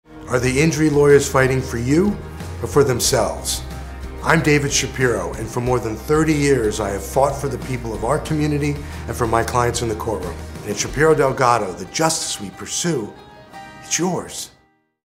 Injury Attorney David Shapiro Fights for Your Financial Rights
Don't trust your injury claim to any law firm. Go with a firm that has not only a track record of success but one with glowing client reviews. Sarasota personal injury attorney David Shapiro has been fighting for the financial rights of accident injury victims for over 30 years.